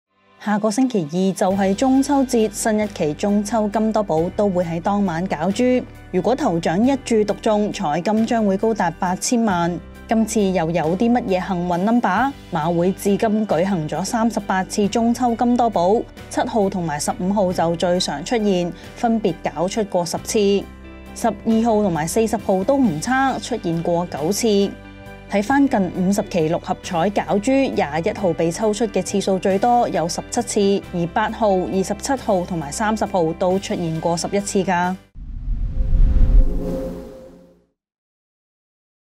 六合彩︱中秋金多寶9.17攪珠 頭獎一注獨中8000萬 即睇最旺號碼！｜星島頭條新聞｜馬會｜中秋節｜金多寶｜頭獎｜8000萬
六合彩中秋金多寶將於下周二（17日）攪珠，頭獎一注獨中彩金高達8,000萬，市民可於今晚六合彩（12日）攪珠後，在各場外投注處、電話投注（包括1886電話投注自動服務系統）及數碼平台購買。